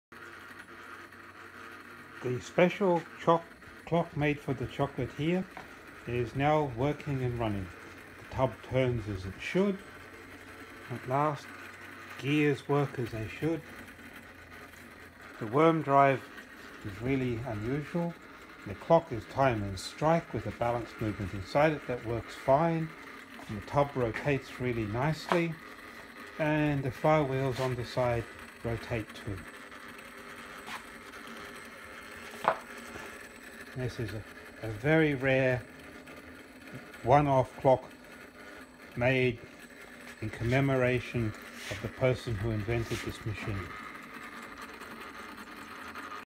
Turbine a Dragee
A special one-off Industrial automata created for M. Leclaire who invented this machine to chocolate coat sugar candies